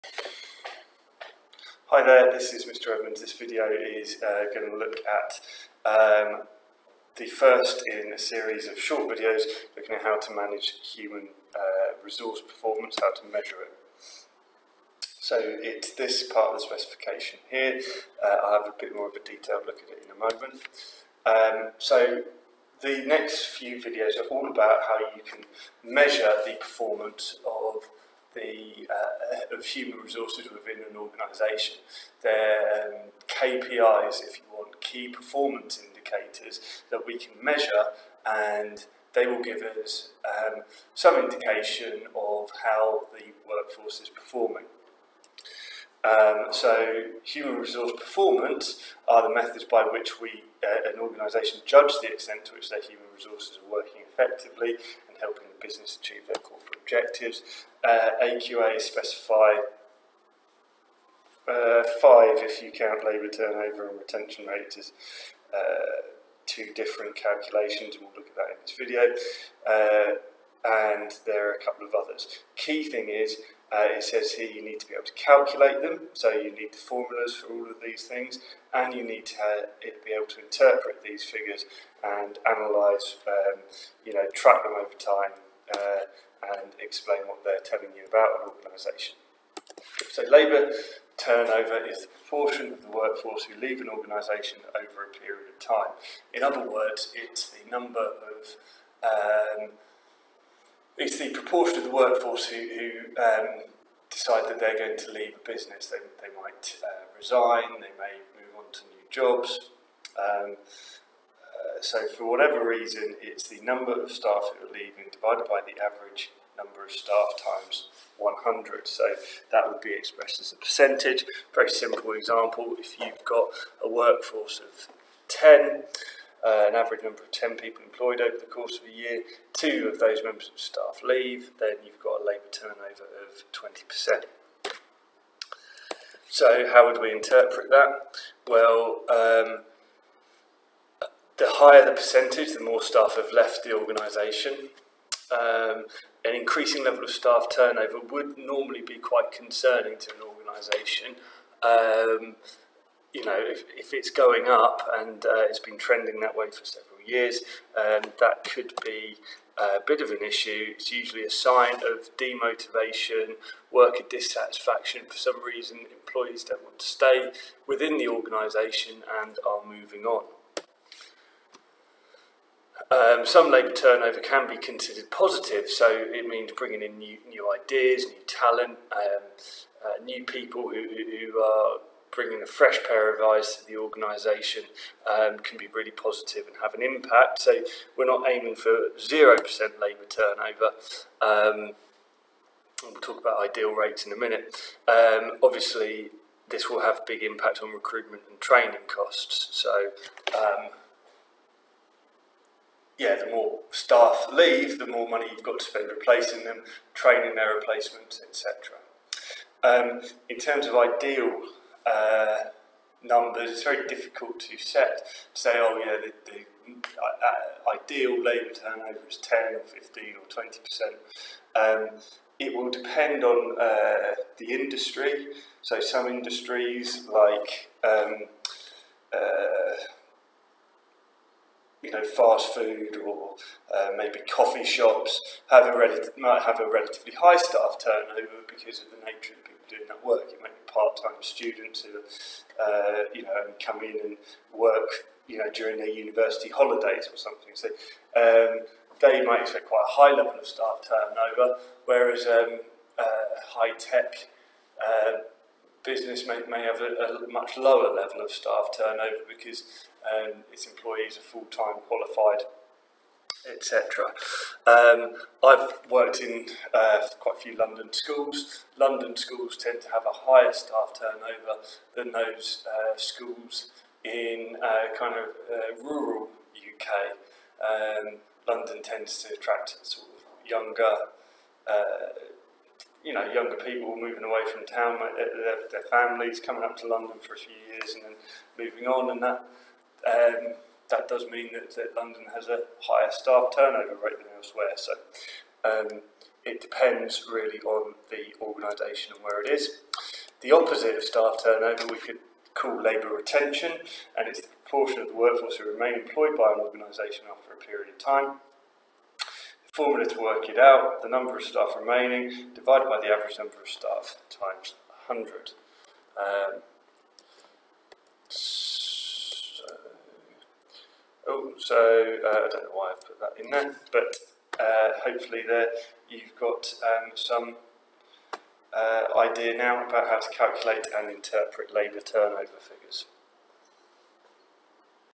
3.6 6 Labour turnover
This video looks at how to calculate and interpret labour turnover.